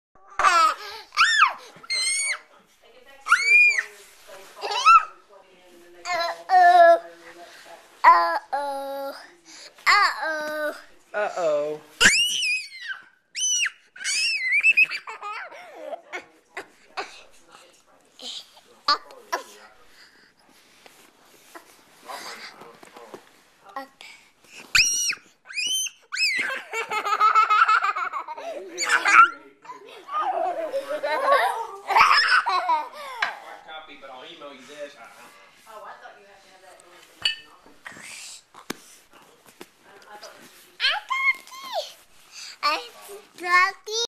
Judah resting (?) between playing with Momma and Dada
Recorded on January 8, 2010 using an HD Flip Video camcorder.